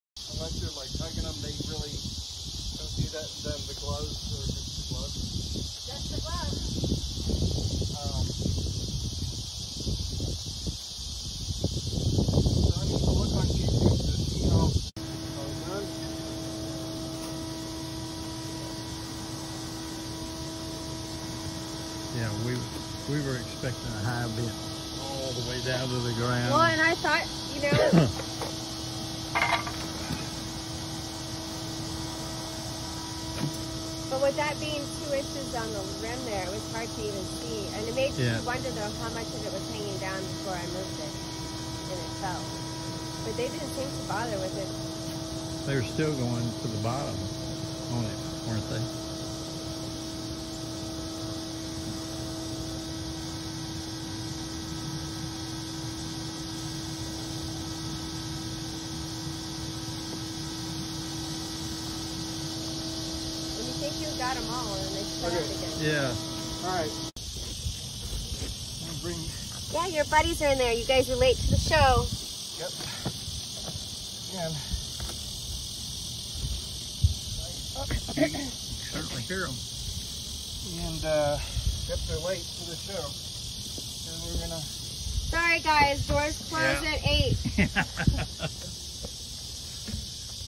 Bee Vacuum
Using a home-made bee vacuum to extract a colony from a water meter pit.